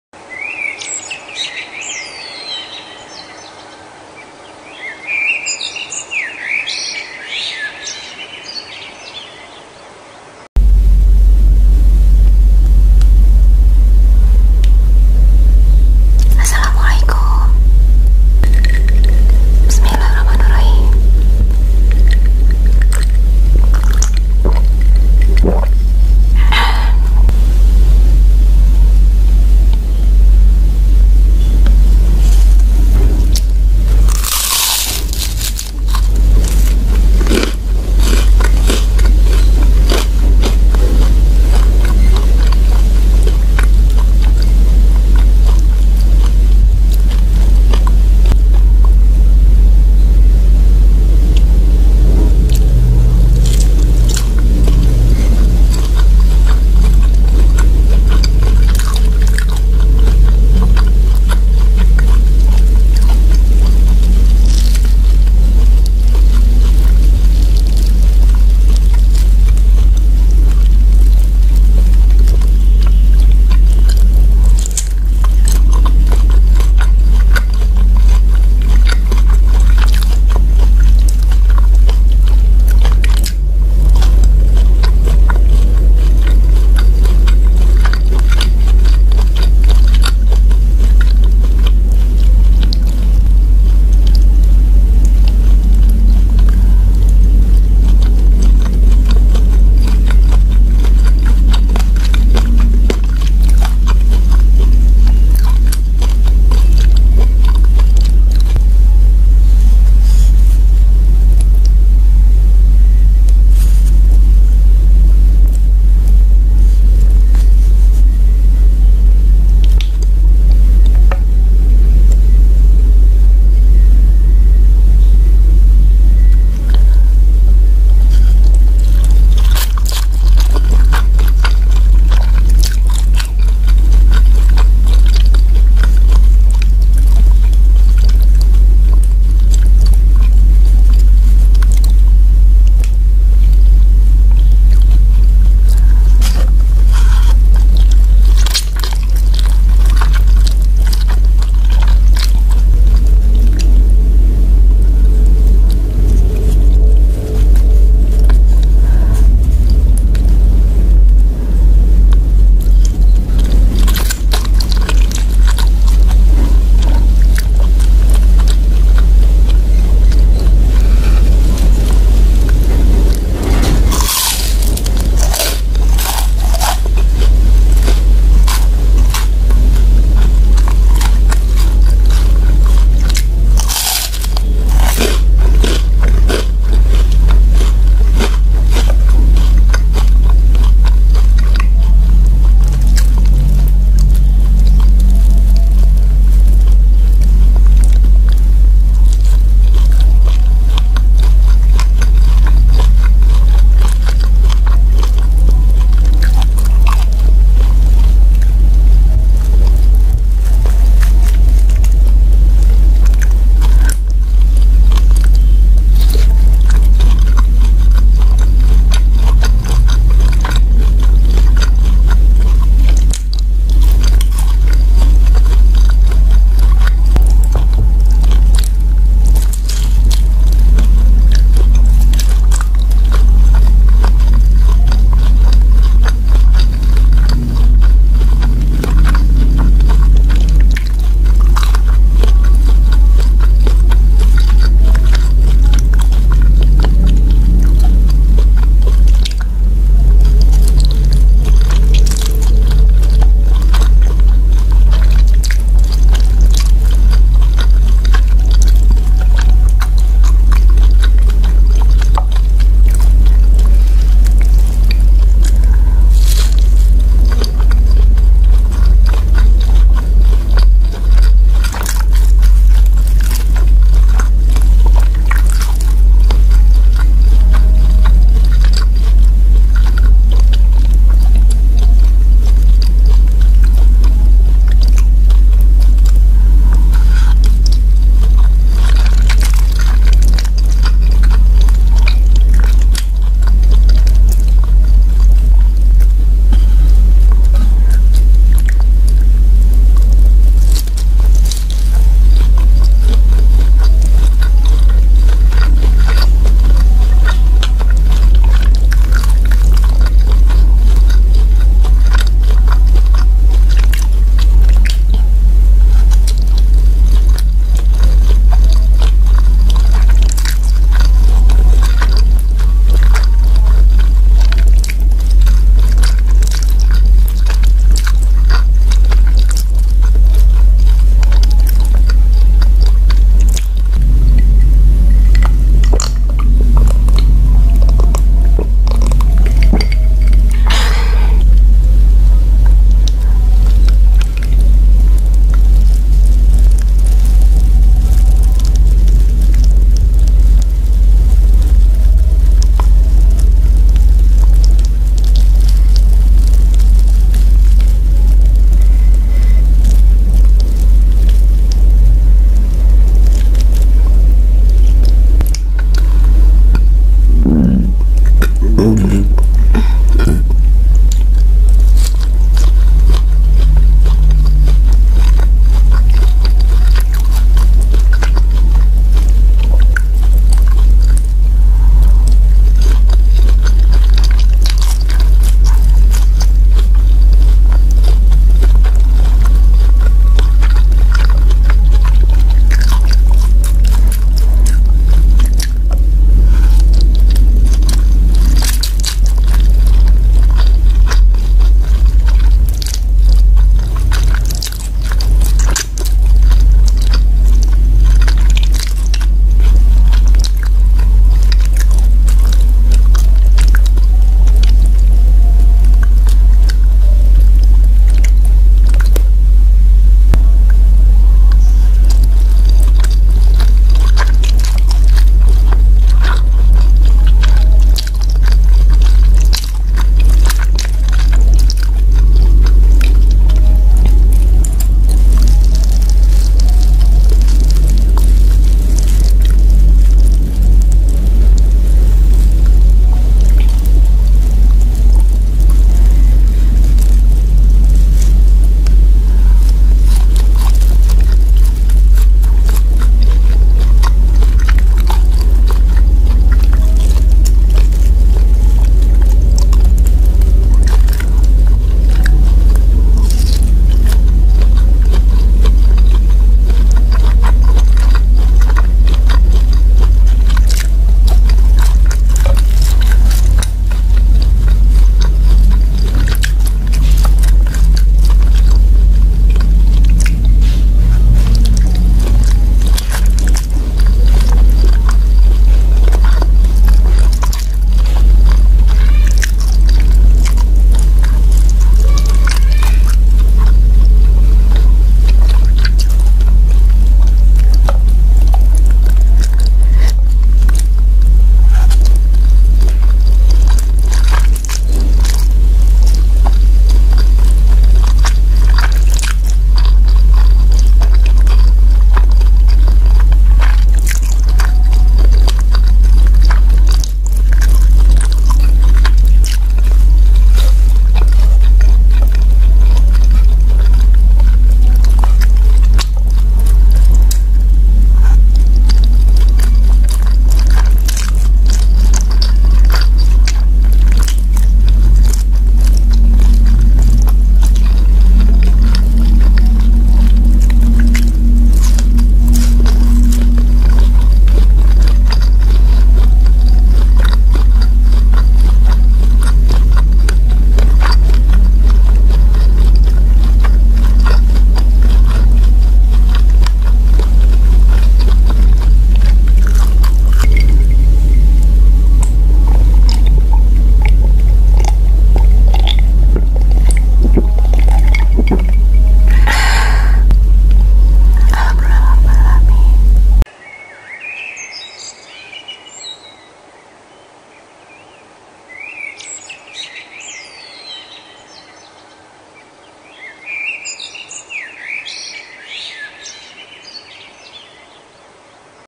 ASMR 🍜 MIE SAMYANG BAKSO MERCON + JUS JERUK 🍊| ASMR INDONESIA | MUKBANG INDONESIA | Eating Sounds
Halo teman teman youtube
Selamat datang di Chanel Youtube saya
ASMR ini adalah video suara makan
Konten video yang saya sajikan adalah video makan bersama saya dengan berbagai menu yang saya sukai
Dalam video ini, saya makan mie samyang korea dengan bakso urat jumbo yang super pedas, minuman jus jeruk.
(Autonomous Sensory Meridian Response) adalah sensasi positif dan menenangkan. Perasaan yang menyenangkan dan saya harap video ini membuat teman-teman relax

Taken by : Redmi Note 9